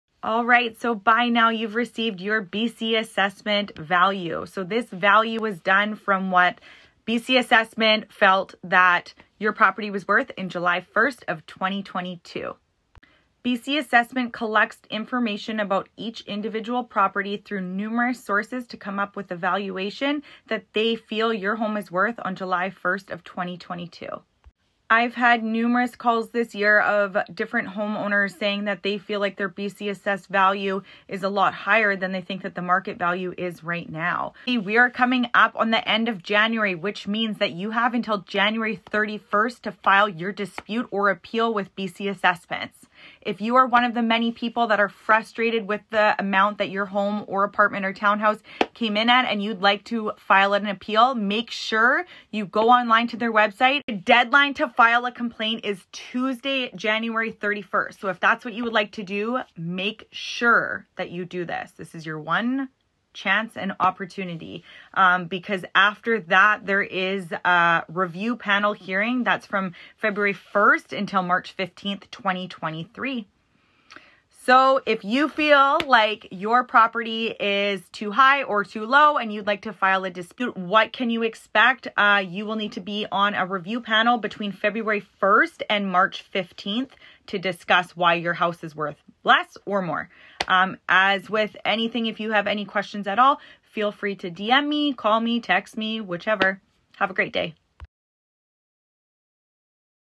POTENTIAL TO SAVE $$ - BC ASSESSMENT
💡 Did you know that you can dispute/appeal your BC Assessment?? If this is your plan, hurry because the cut off is January 31!